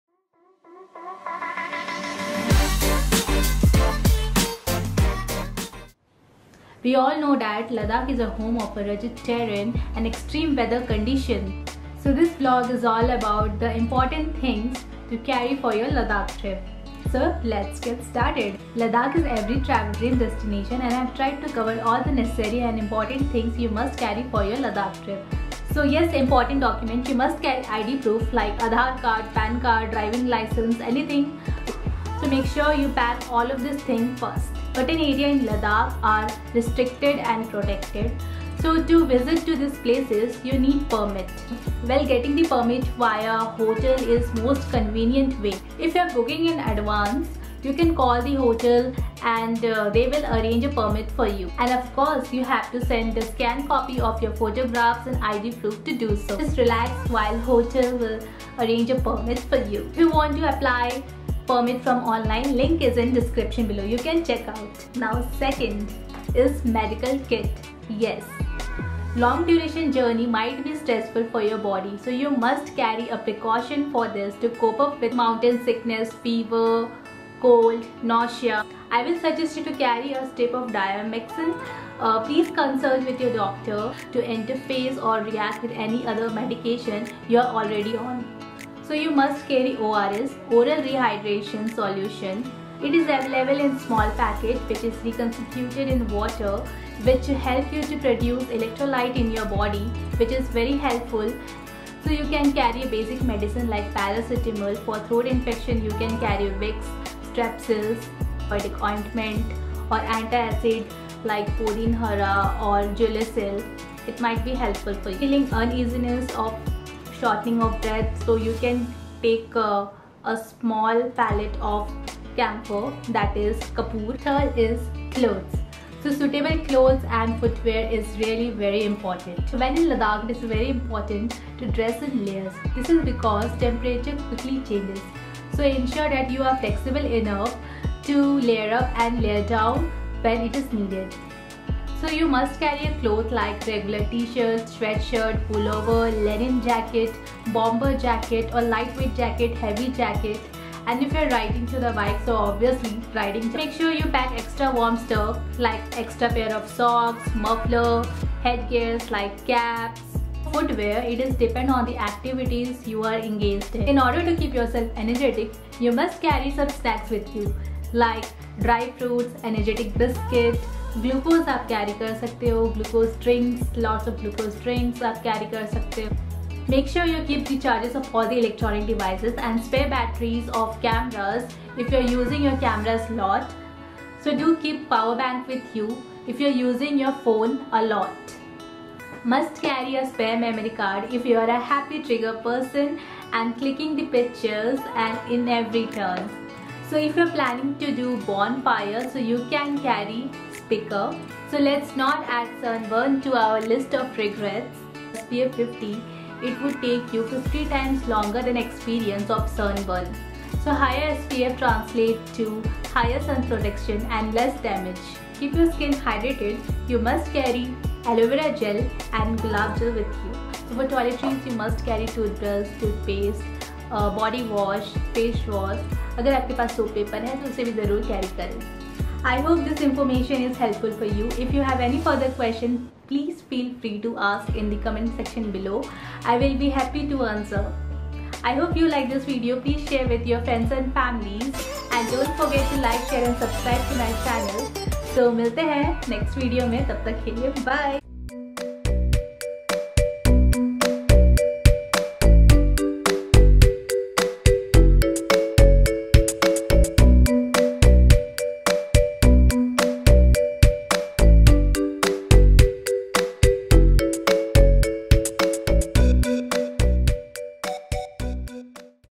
How to prepare and things to Carry for Ladakh Road Trip
This Vlog is all about the essentials things you must carry for your Ladakh Trip.
Ladakh is every traveller's dream destination.
Here is the Quick Overview of Packing List:
1) Documents
ID Proof
 Permits - ILP
Vehicle Documents
Mobile connectivity
only BSNL Postpaid Sim works here.
2) Medicines
Diamox -  reduce the symptoms of altitude sickness. This medication can decrease headache, tiredness, nausea, dizziness
ORS (Oral rehydration solutions) treat the loss of too much body water dehydration.
Basics Medicines like Paracetomol ,Throat infection -vicks cough drops or strepsils
 Minor Wounds- Betadine or Iodine disinfection Solution
 Bandaids, Anitibiotic Ointments
Antacids like Pudin hara or Gelusil
A small Vial containing Camphor Pellets. 10-20 bulbs of Raw Garlic and lemon .
3) Clothes
Cardigan /waterproof Jackets/ Rinding Jackets
 3- 4 pairs of Socks
 2 pairs of thermals
Gloves ,Mufflers , Caps
4) Eatables
 Dry fruits, dates
 Chocolates
 Buns and Breads.
Glucose drinks and water.
5) Gadgets
Camera
 Spare Batteries
 Selfie Sticks
 Power Banks
Speakers
6) Skin Care and Toiletries
Suns-cream SPF 50
Moisturisers
Paper Soap
Tissues
Face wash/ Body wash.

I hope this information is Helpful for you for hassle free Trip.